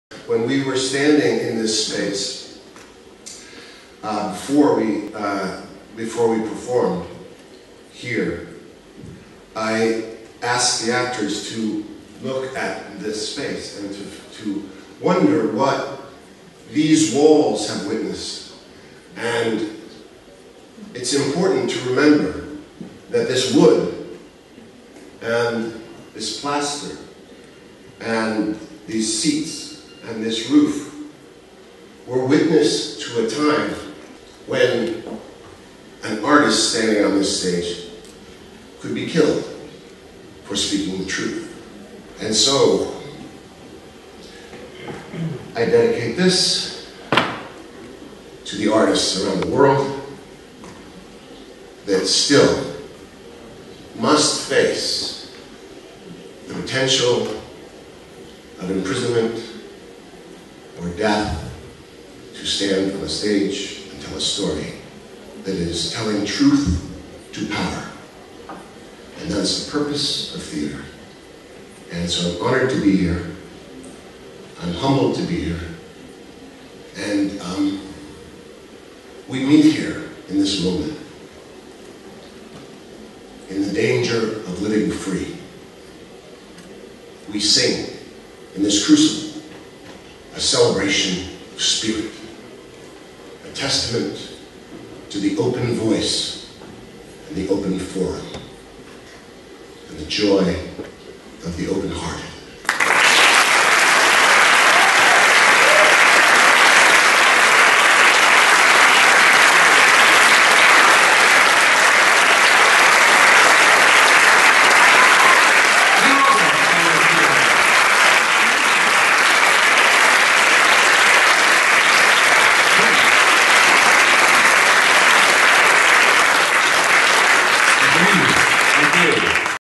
Tim Robbins at the FITS Gala
Mr. Tim Robbins gave an inspirational and touching speech for both artists and common people to keep in mind.